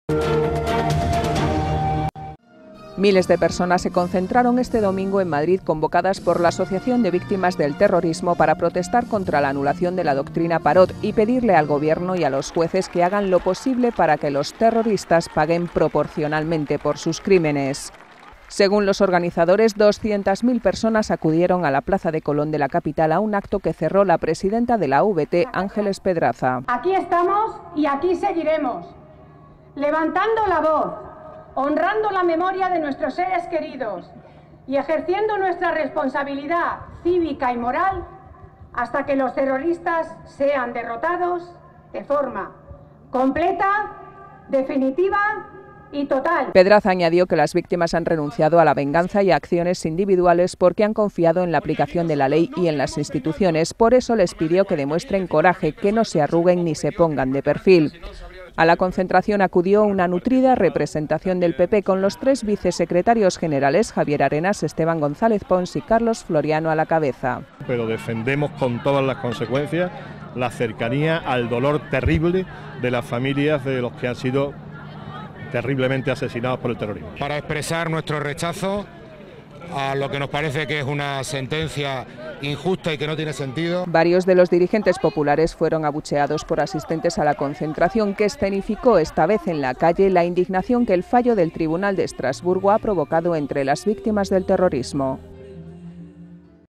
Las víctimas del terrorismo: "Pedimos justicia, no venganza"
Madrid, 27 octubre (CERES TV | EFE)

Las víctimas del terrorismo han pedido hoy al Gobierno y a los jueces que demuestren "coraje" y que "no se arruguen" ni se pongan "de perfil" ante el fin de la doctrina Parot, y les han instado a que hagan lo posible para que lo terroristas paguen proporcionalmente por sus crímenes.

Miles de personas -200.000 según los organizadores, mientras que la Delegación del Gobierno no ha facilitado cifras- se han congregado hoy en la Plaza de Colón de Madrid y sus aledaños, convocados por la Asociación de Víctimas del Terrorismo (AVT), para protestar por el fin de la doctrina Parot y manifestar a las víctimas que no están solas en su lucha.

La presidenta de la AVT, Ángeles Pedraza, ha sido la encargada de cerrar un acto en el que dirigentes del PP han sido increpados por la actitud de este partido y del Gobierno ante la resolución del Tribunal Europeo de Derechos Humanos (TEDH) que anuló la doctrina Parot.

Entre ellos los tres vicesecretarios generales del PP, Javier Arenas, Esteban González Pons y Carlos Floriano, así como diputados nacionales que han sido abucheados por algunos de los asistentes, que les han llamado "traidores" y "cínicos" y que han echado de menos al presidente del Gobierno, Mariano Rajoy.

Junto a ellos, la ex presidenta de la Comunidad de Madrid Esperanza Aguirre; su sucesor en el cargo, Ignacio González; la presidenta de Navarra, Yolanda Barcina; la presidenta del PP en el País Vasco, Arantza Quiroga, o la alcaldesa de Madrid, Ana Botella.

Durante su discurso, y ante algunos de estos abucheos, Pedraza ha pedido respeto para las víctimas y ha solicitado la aplicación de la ley, "pero de toda la ley" y de la justicia, "pero de toda la justicia", a la vez que ha exigido a los jueces que estén a la altura de los sacrificios de las víctimas.

Ha citado expresamente al magistrado español Luis López Guerra, que votó a favor de la derogación de la doctrina Parot en la Corte Europea, como uno de los que no han estado a esa altura.

En un estrado presidido por una gran pancarta con la palabra "Justicia", flanqueada por otras dos con sendos lazos negros, la presidenta de la AVT se ha rodeado de víctimas de los etarras Henri Parot (cuyo caso dio nombre a la doctrina) e Inés del Río, que ganó el recurso ante Estrasburgo y ya está en libertad, para escenificar el dolor de los afectados por el terrorismo.